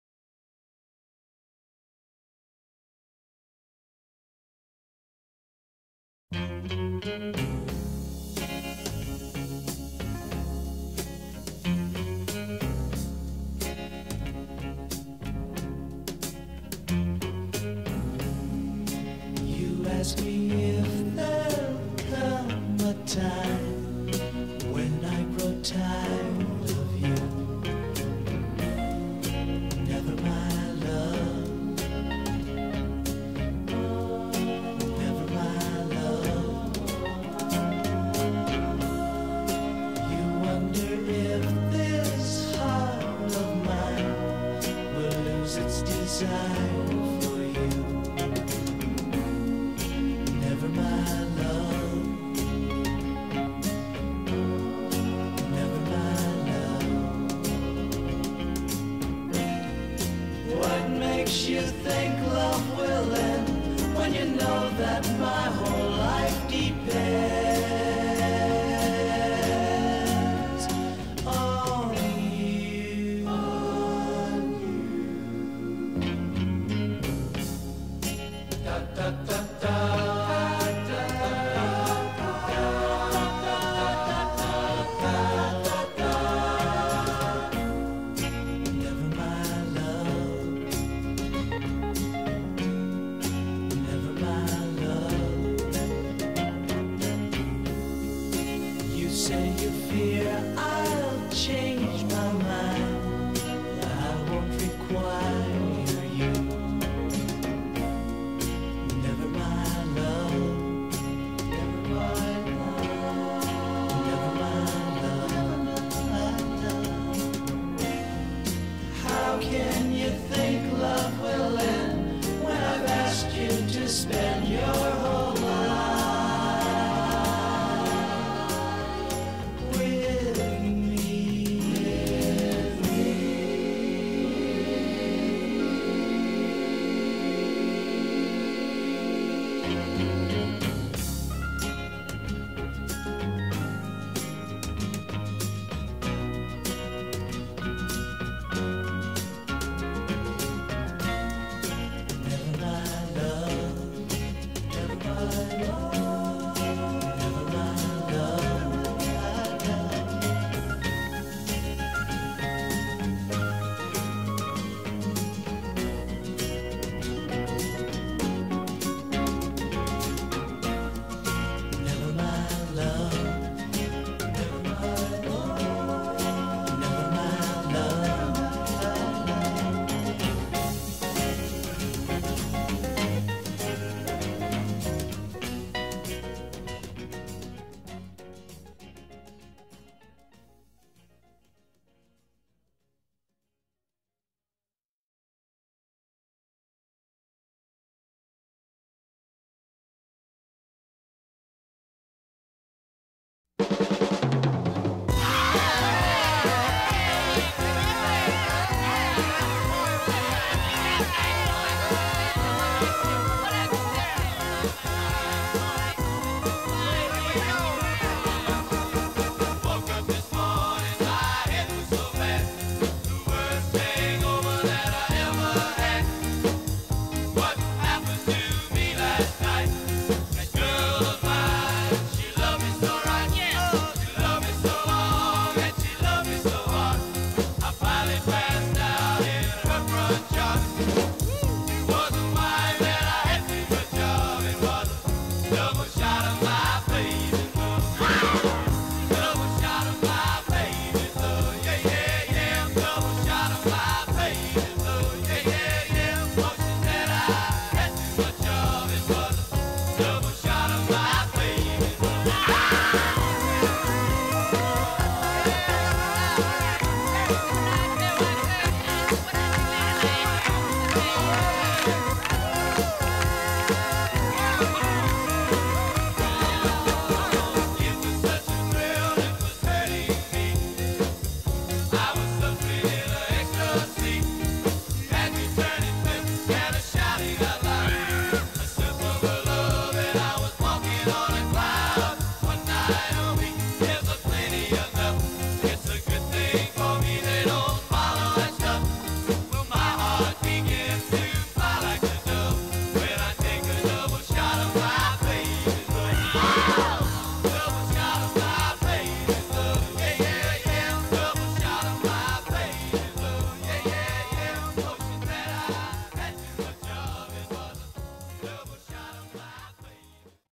Skiing Vail 1965
Brad and Caroline McKee skiing Vail in 1965 with friends the Hollerbachs. A few quick shots of Pepi and Jean Claude Killy at the end. Clear Creek canyon and early Vail, Colorado.